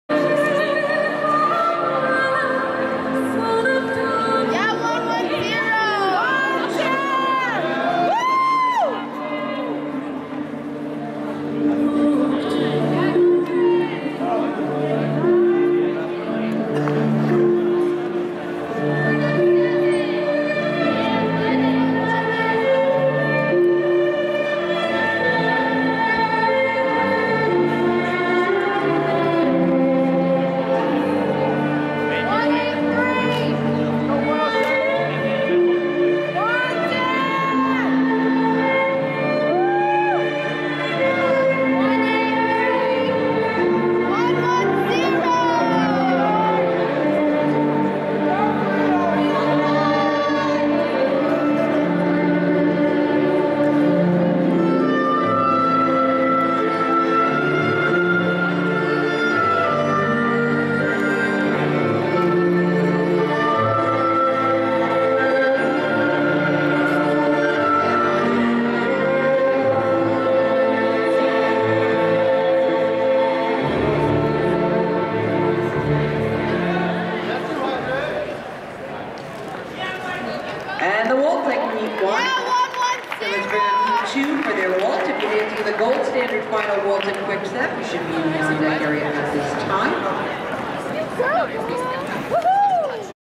OSB 2013 Bronze Standard Waltz Round 3
Bronze Standard Waltz round 3. couples: Elena Segre Leading Yana Morenko